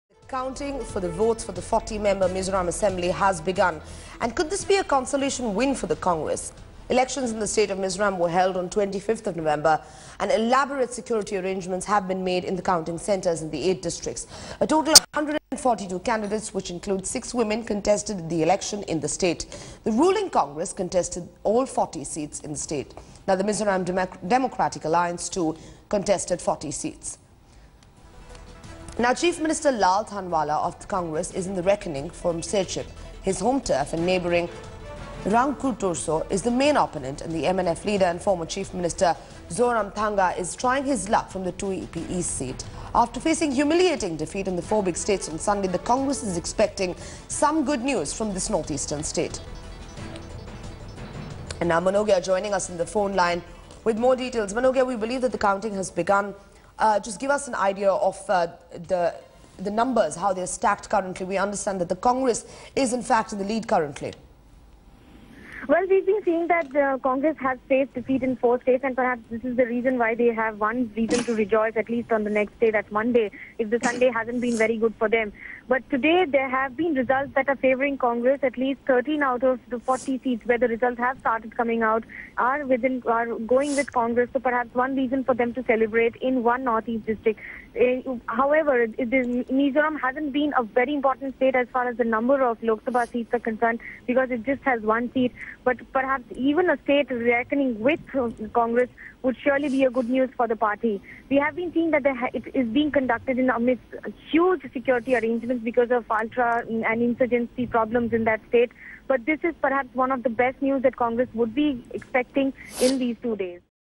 Counting begins for Mizoram assembly polls
Counting of votes for the 40-seat Mizoram Assembly polls has begun.